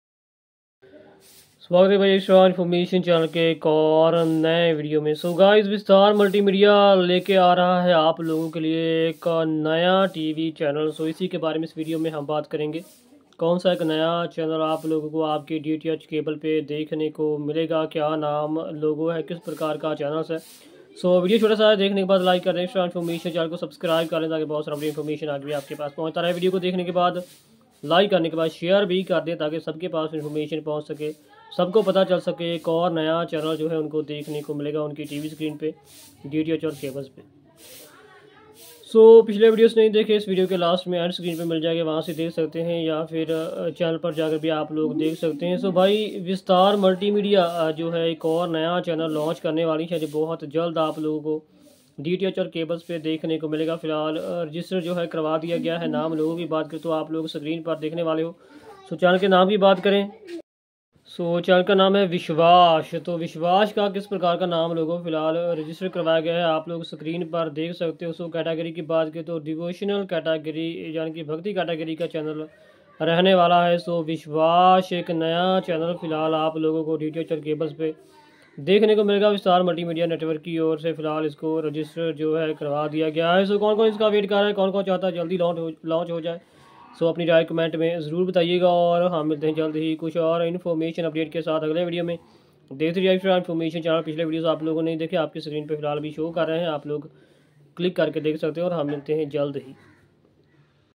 1 NEW CHANNEL LAUNCHING ON TATA PLAY AIRTEL DIGITAL TV DISH TV D2H AND DD FREE DISH 24 November 2024
1 NEW CHANNEL LAUNCHING ON TATA PLAY AIRTEL DIGITAL TV DISH TV D2H AND DD FREE DISH || 24 November 2024

Hello friends

Vishwash TV
Upcoming Channel

Channel host:
Gurbaj Singh

Note : 🙏

Enjoy Sunday